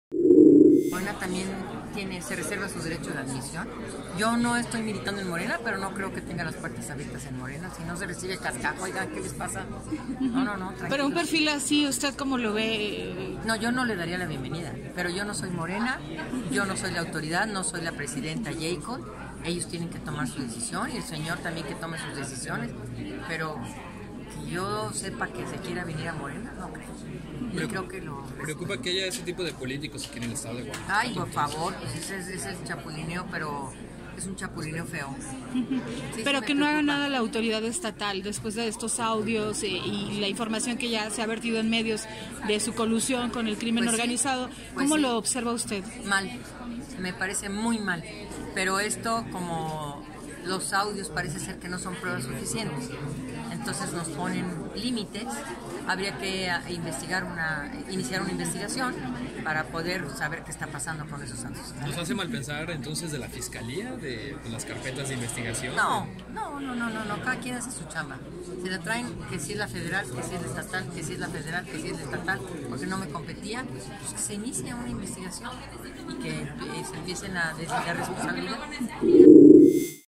En Morena no se reciben ‘cascajos’
Después de que el exdirigente estatal del PRD, Hugo Estefania Monroy, anunció su salida de dicho partido político, la senadora por Morena, Malú Micher, dijo que en Morena se reservan su derecho de admisión, pues descartan que se le reciba como nuevo militante.